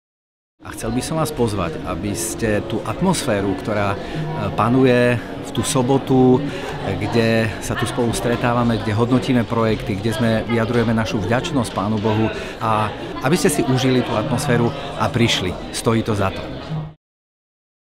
Pozvánka na unijní setkání
Předseda ČS Unie CASD Mikuláš Pavlík vás zve na příští unijní setkání ASI-CS. Činí tak přímo ze 14. unijního setkání v Olomouci 7. prosince 2018.